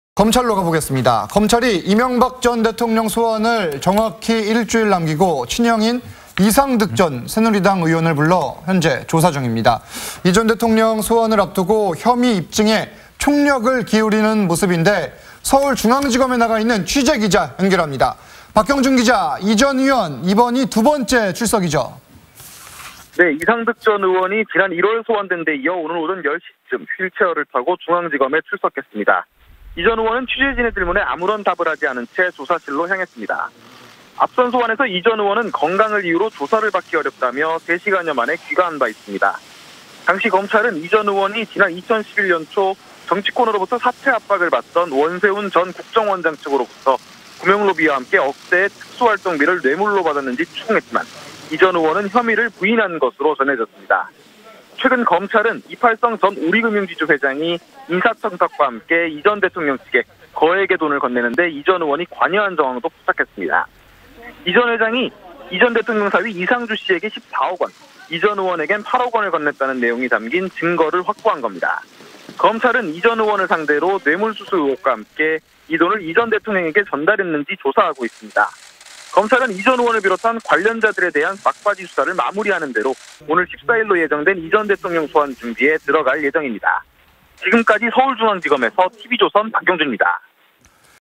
'불법자금 수수' 이상득 검찰 재출석…이명박 소환 전 막판 수사
검찰이 이명박 전 대통령 소환을 1주일 남기고, 친형인 이상득 전 새누리당 의원을 불러 조사하고 있습니다. 이 전 대통령 소환을 앞두고 혐의 입증에 총력을 기울이는 모습입니다.